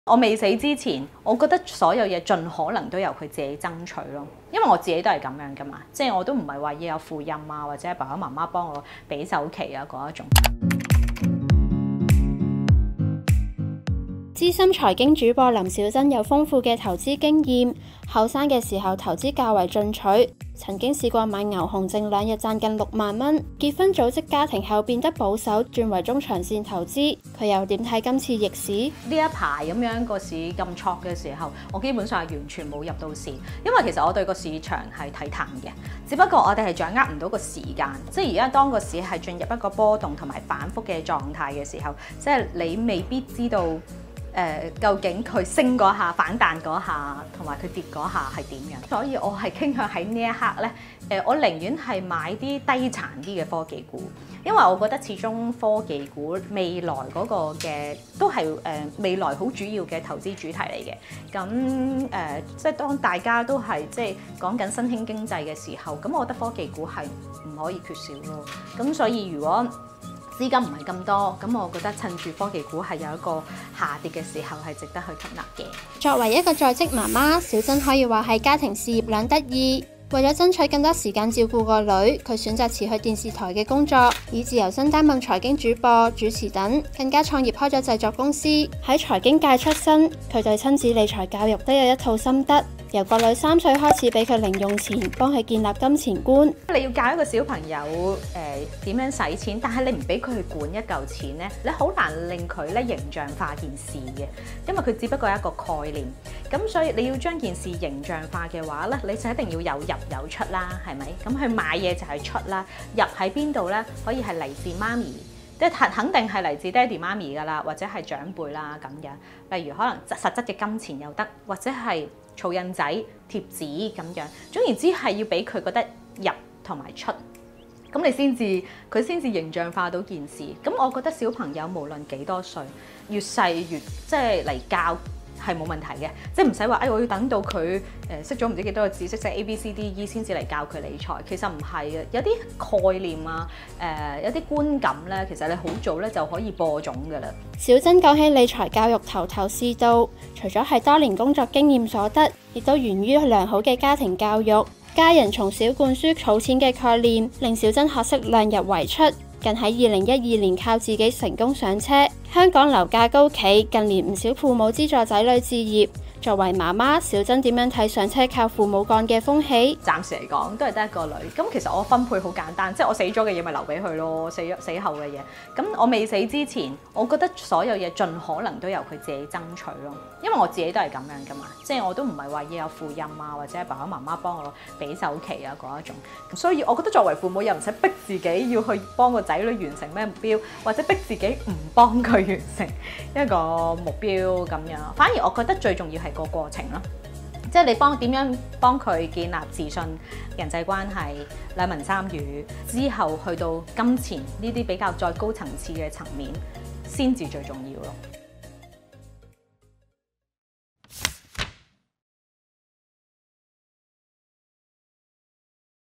財經主播林小珍談理財教育：昐女兒靠雙手打拼（2020年5月15日）
【香港經濟日報】資深財經主播林小珍（June）早年為爭取時間照顧女兒而辭去電視台的工作，現轉職自由身續擔任財經主播、主持及司儀等工作。作為財經新聞界出身的媽媽，她會否期望女兒將來不會依靠「父母幹」，現時又如何為年幼的女兒建立理財觀？…（更多內容：